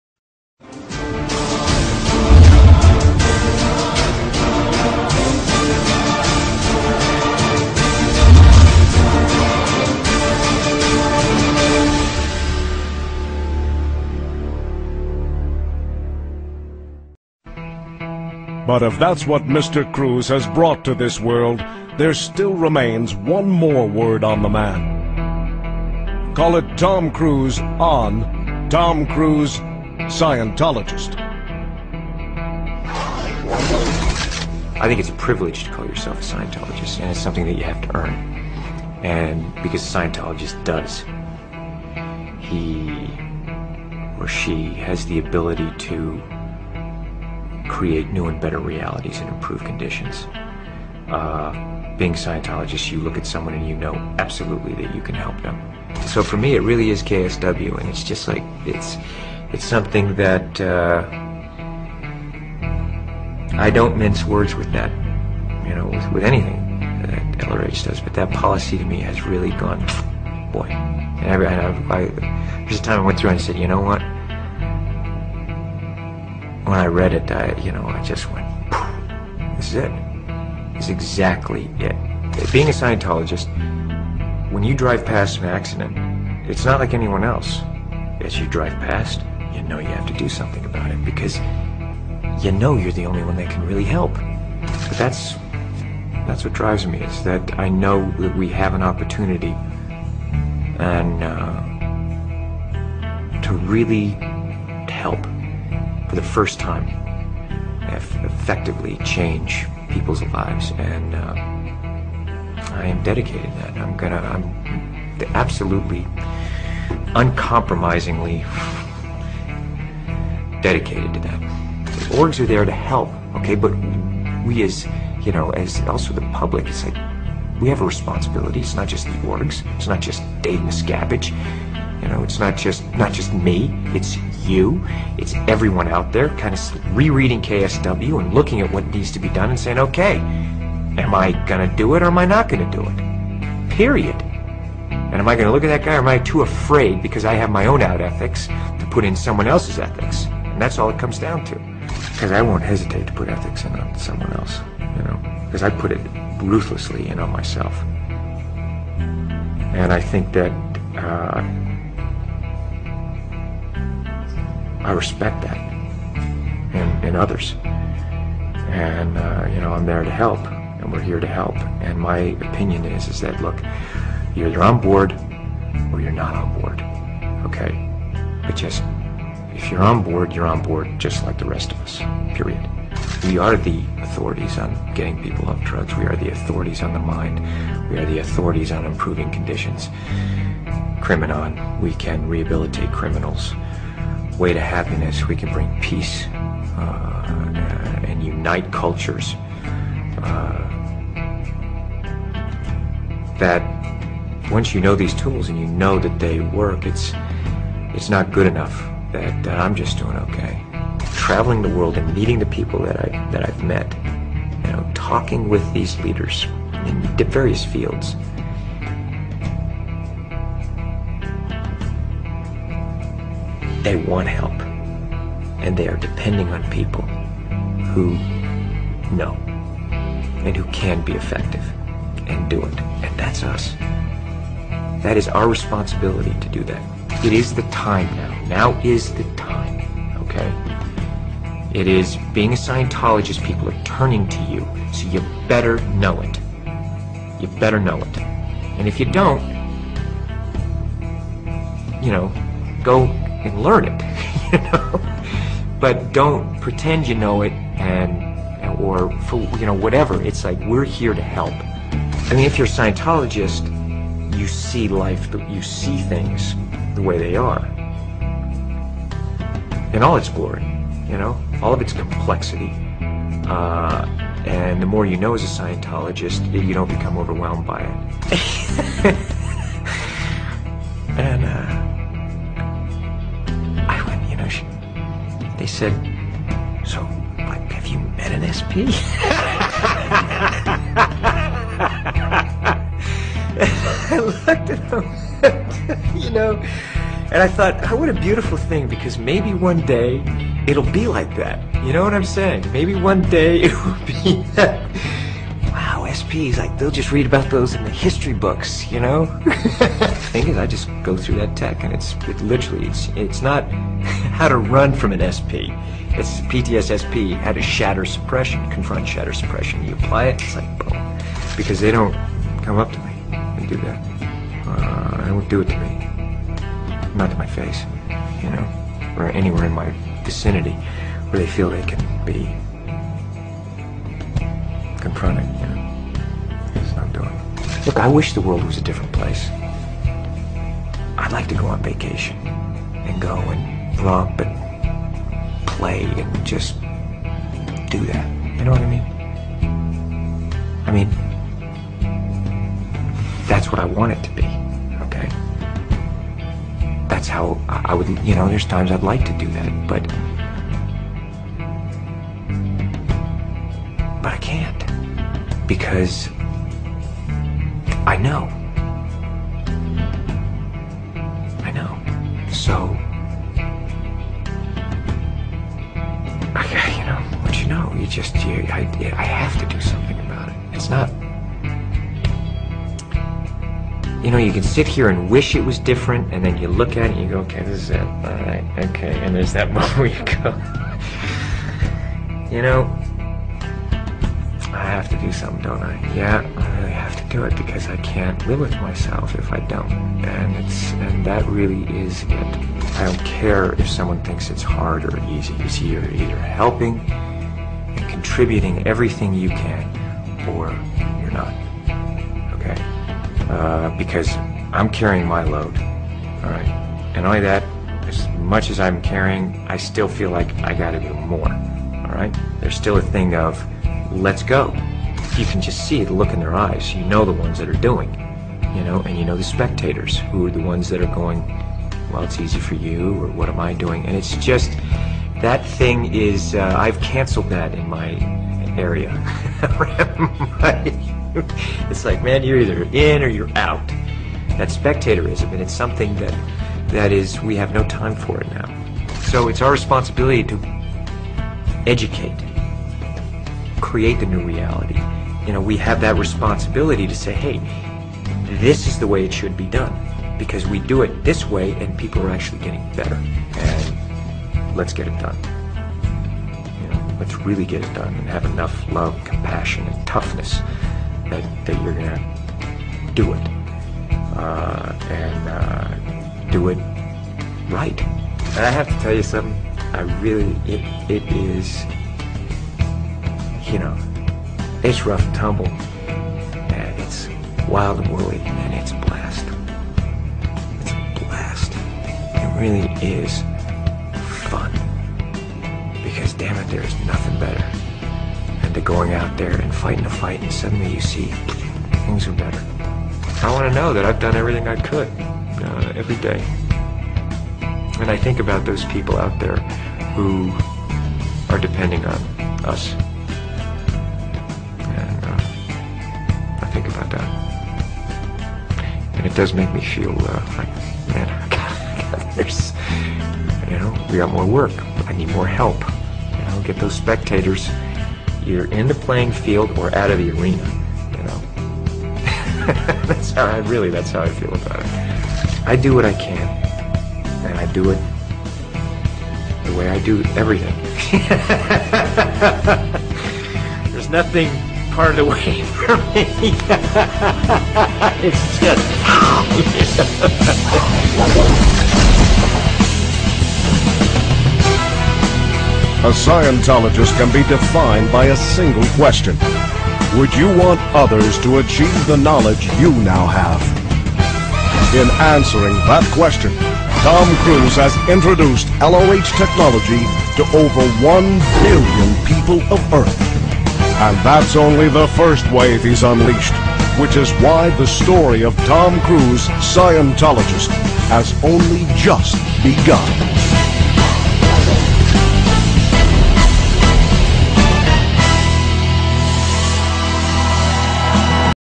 Anonymous German vs Scientology
Anonymous German     Der Deutsche Verfassungsschutz ist auch gegen Scientology

Wir sind Anonymous
Wir sind eine Legion
Wir vergessen nicht
Wir vergeben nicht
Erwartet uns!